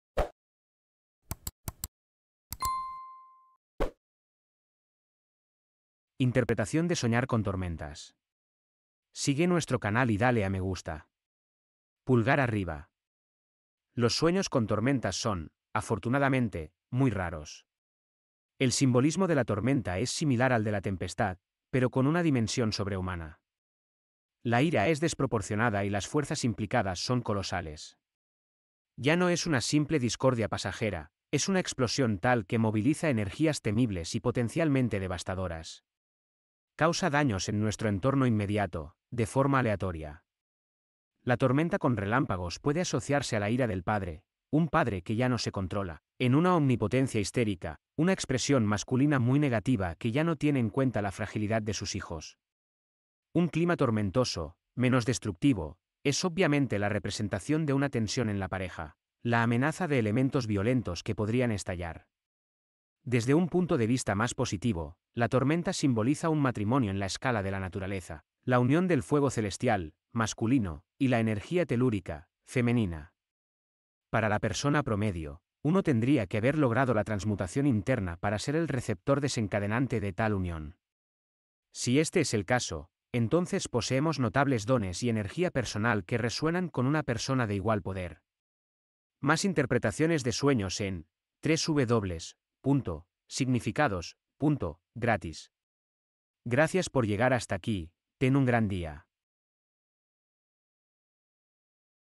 ¿QUÉ SIGNIFICA SOÑAR CON TORMENTAS?
¿Has soñado con tormentas? Conoce la interpretación, el significado de soñar con tormentas.